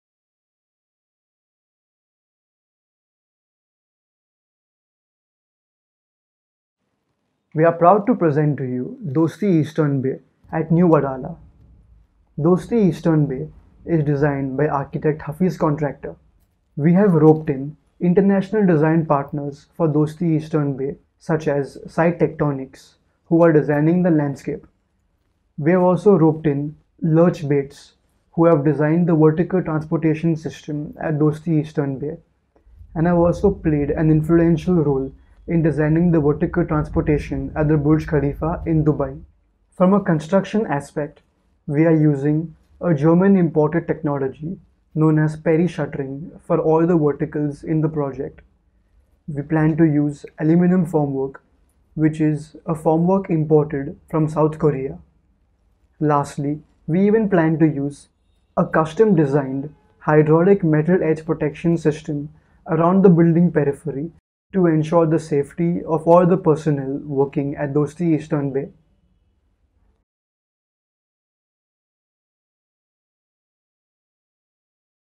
Redefining New Wadala’s Skyline. - Mr.Anuj Goradia, Managing Director
Watch our Managing Director, Mr. Anuj Goradia on launching the new Luxury Project, towering 44 floors with Luxury Lifestyle and Amenities -Dosti Eastern Bay, New Wadala